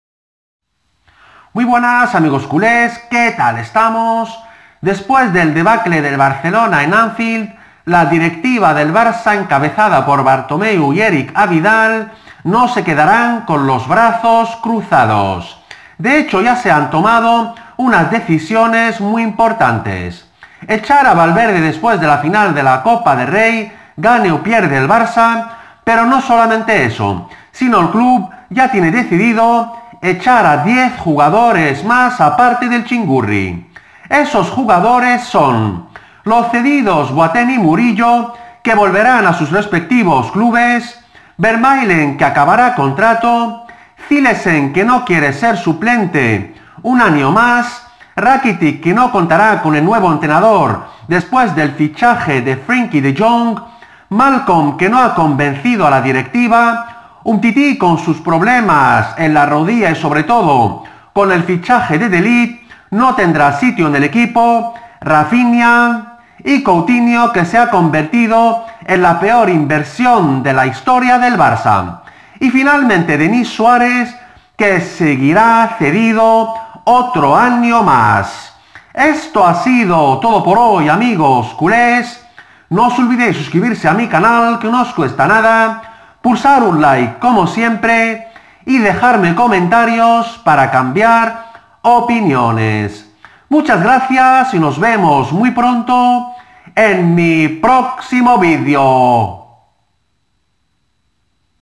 ÚLTIMA HORA: ¡¡EL BARCELONA ECHA A VALVERDE Y A ESTOS 10 JUGADORES - EMPIEZA LA REVOLUCIÓN!!
El Barcelona después del debacle de Anfield decide cesar a Valverde y echar a díez jugadores más